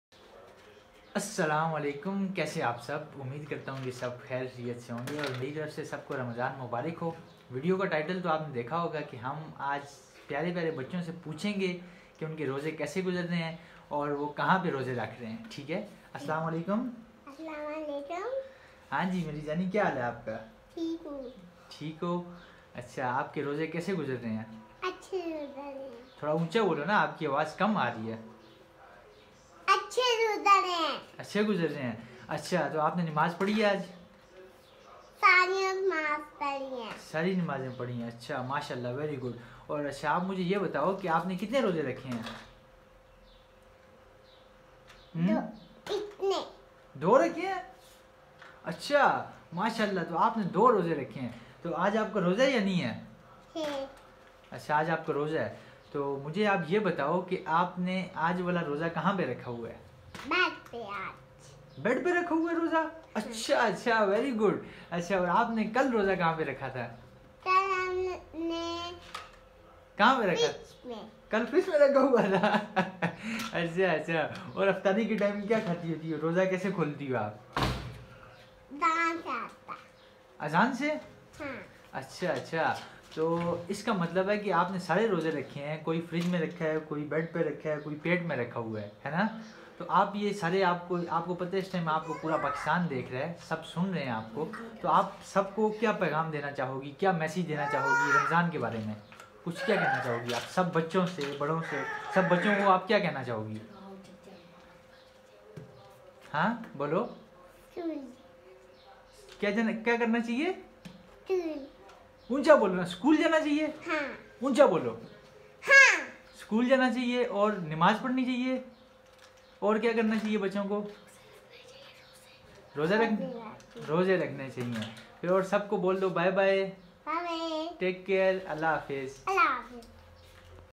Asking from kids About RAMADAN | kitnay rozay rakhay hen | Aj roza kahan rakha ha funniest reply
Q&A with kids About RAMADAN | kitnay rozay rakhay hen | Aj roza kahan rakha ha funniest reply